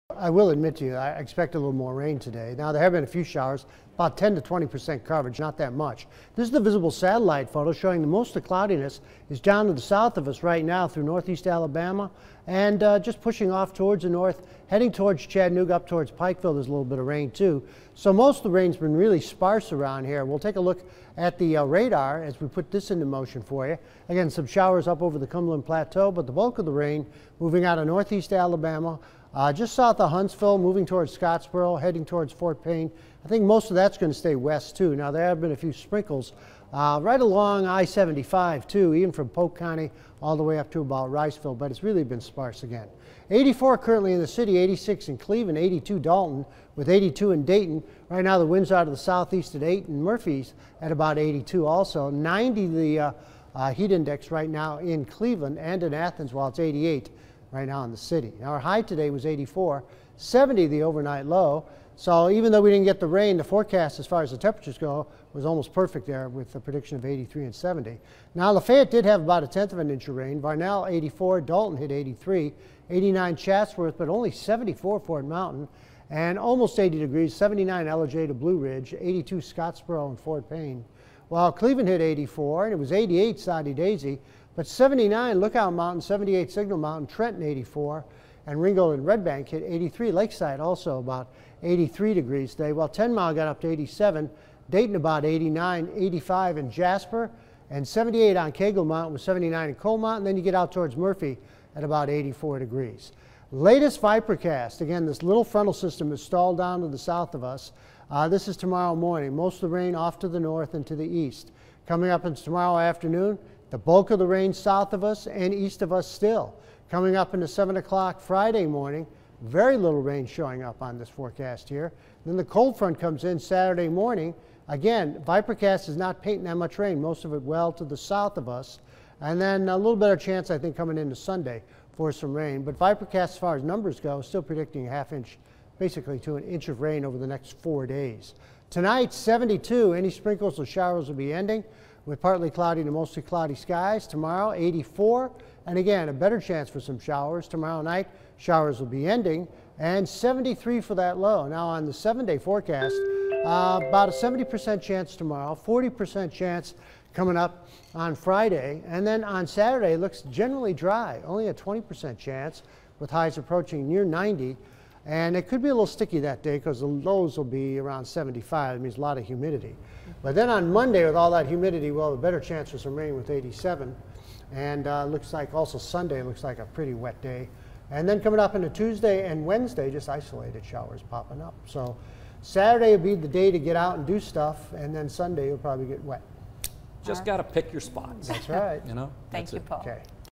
Cloudy tonight, showers expected Thursday
We will continue in an unsettled weather pattern for the next 7 days. Humidity levels will remain high and temperatures will remain below normal and in the 80's. Lows will be in the low to mid 70's.